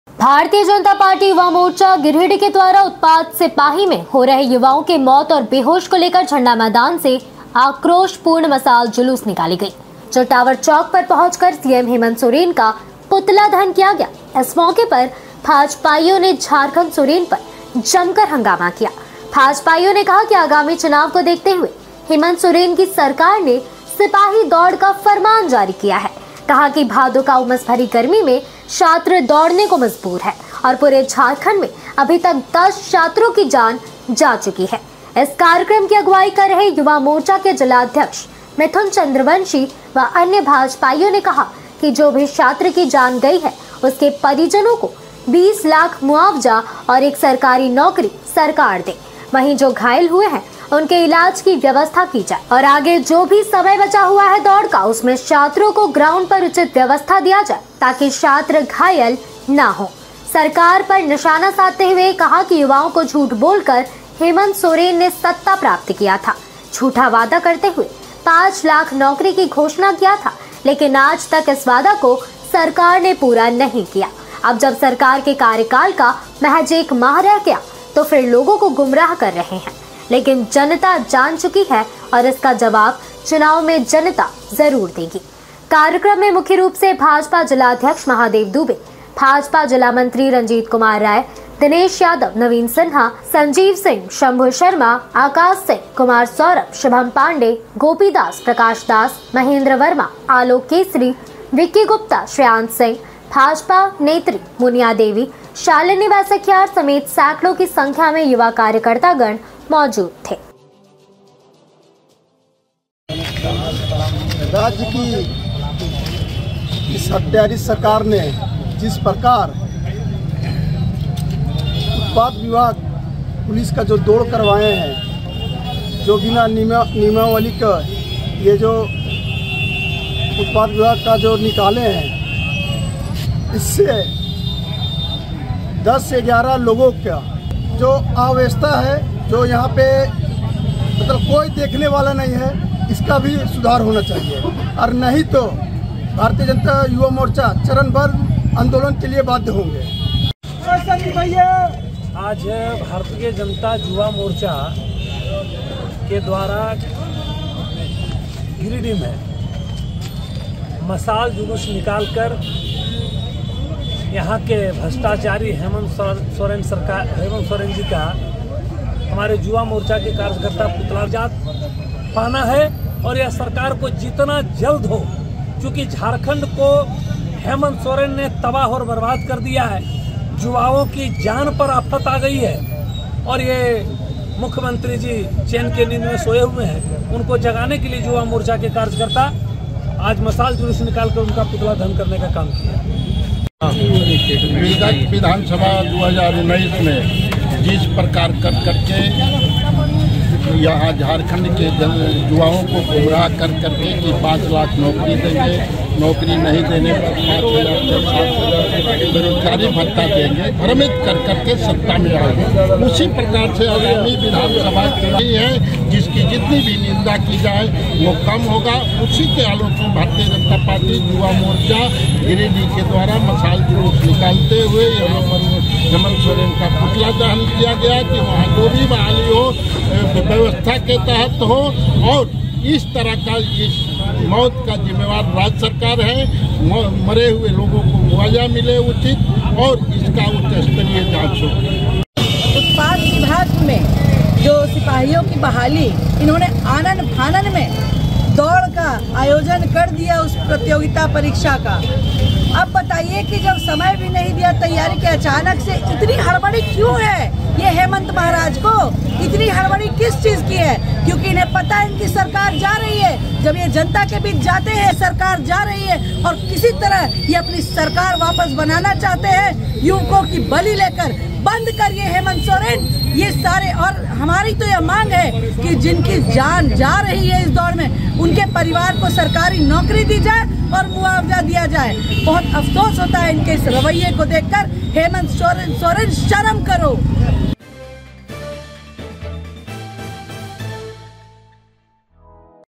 उत्पाद सिपाही दौड़ में हो रहे युवाओं के मौ*त और बेहोशी को लेकर भाजपायुवा मोर्चा ने निकाला मसाल जुलूस
उत्पाद सिपाही दौड़ में हो रहे युवाओं के मौ*त और बेहोशी को लेकर भारतीय जनता पार्टी युवा मोर्चा ने निकाला मसाल जुलूस